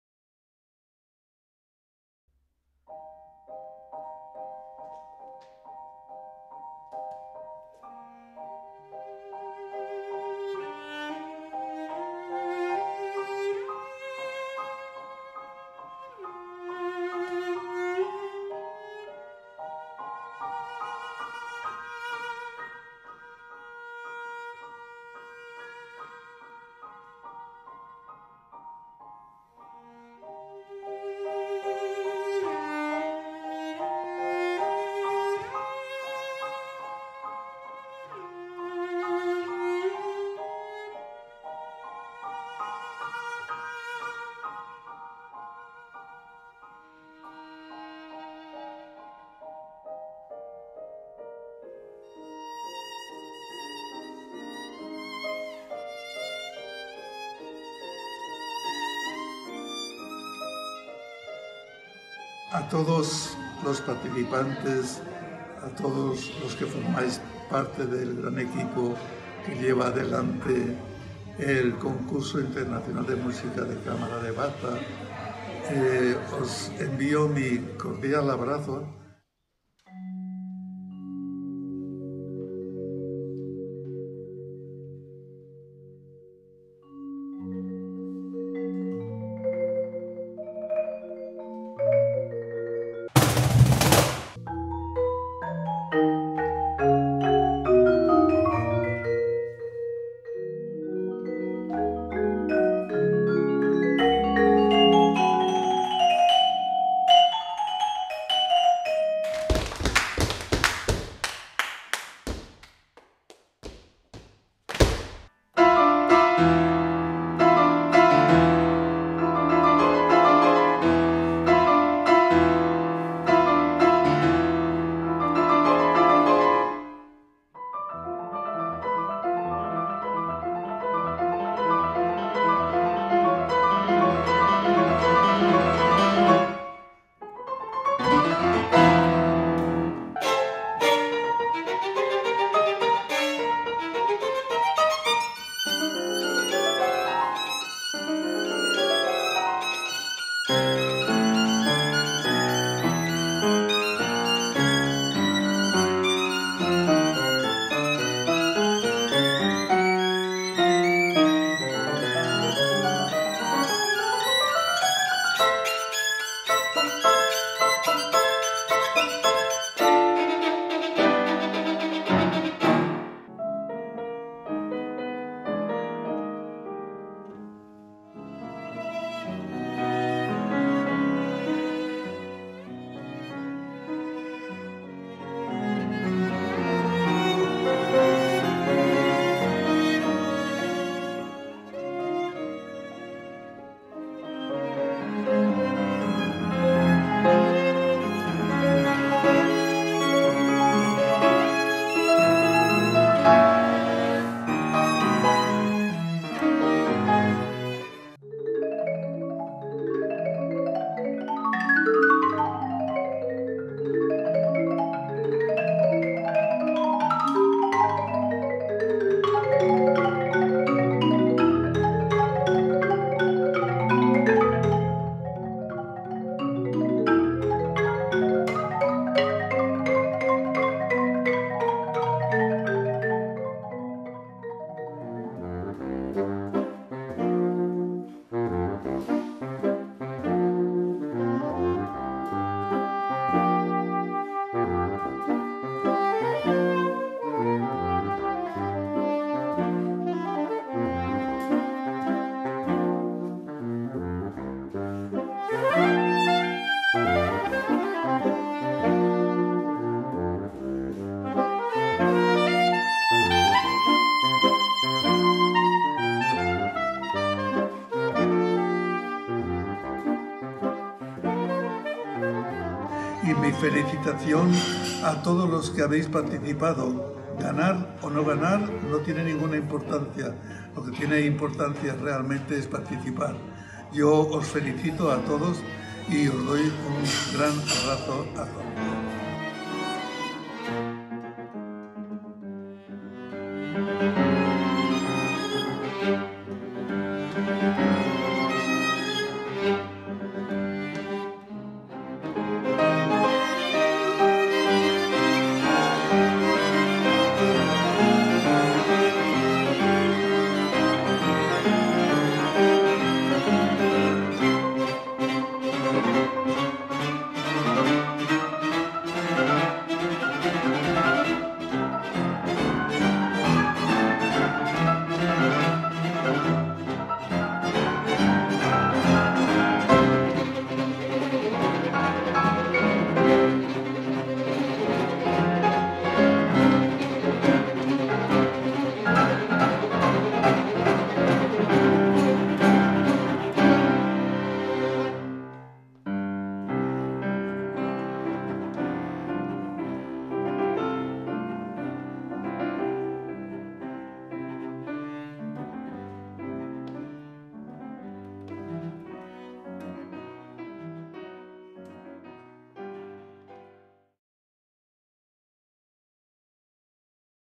VII ANTON GARCÍA ABRIL INTERNATIONAL COMPETITION - 2018
Antón García Abril International Chamber Music Competition 2018. Promotional video. Finalist and winners groups.
Next edition, September 2019